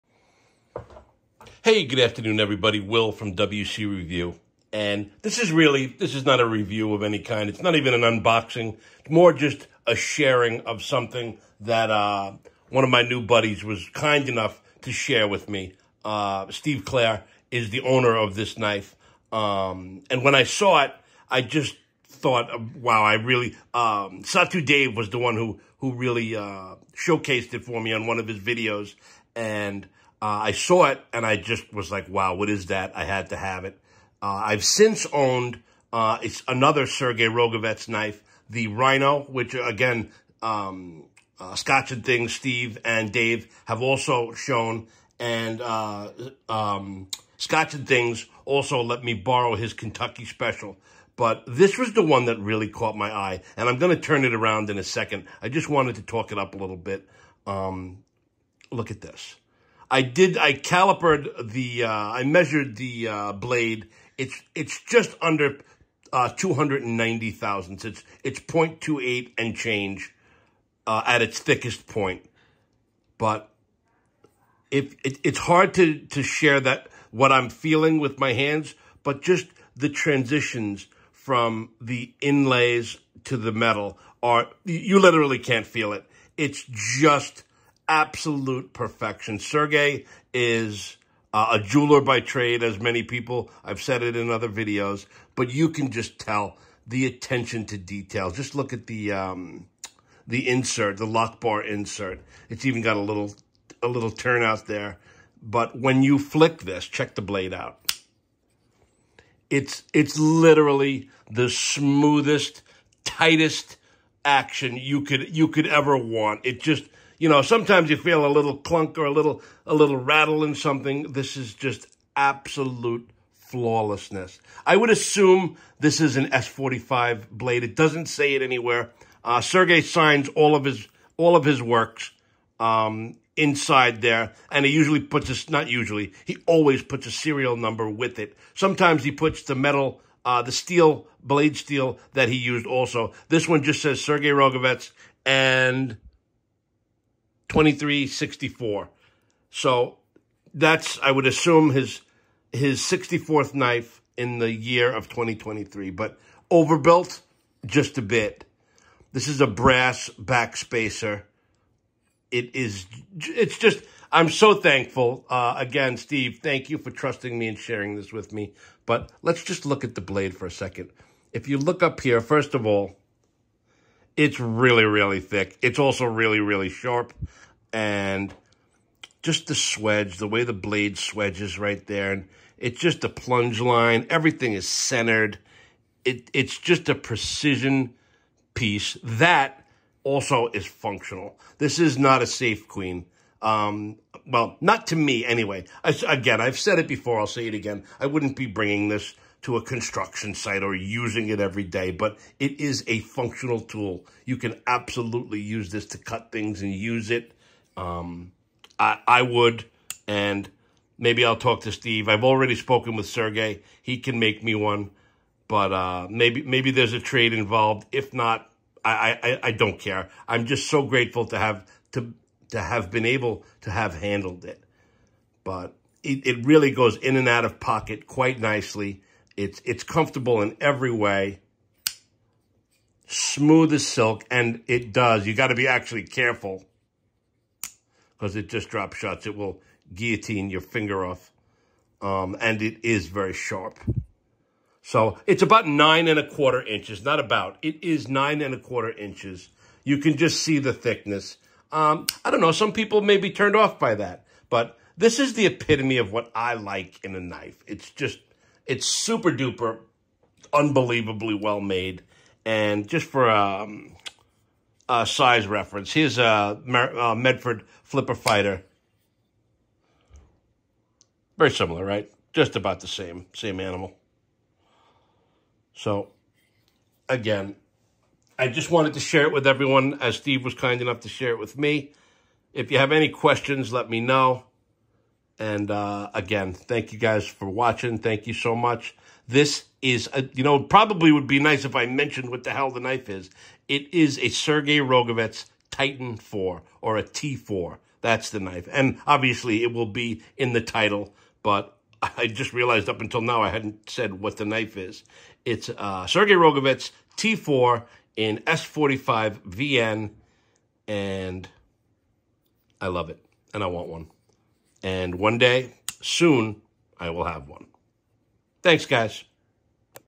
Sergey Rogovets Titan 4 T4 knife
This is as much a work of art as it is a functional cutting tool! I also failed to mention that the finger choil is perfect and my fat fingers fit it it perfectly!